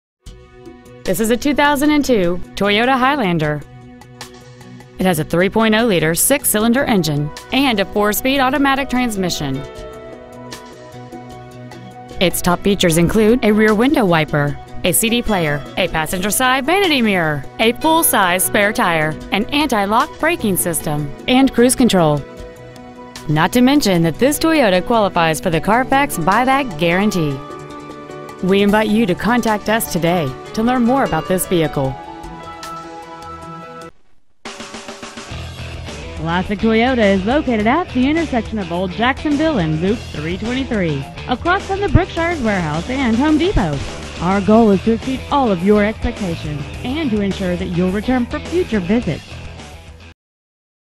2002 Toyota Highlander Tyler TX 75701
Year : 2002

 Make : Toyota

 Model : Highlander

 Engine : Gas V6 3.0L/183

 Trans . : Automatic

 Exterior : Millenium Silver Metallic

 Miles : 117,665

 Interior : Gray

 1717 West SW Loop 323

 Preowned 2002 Toyota Highlander Tyler TX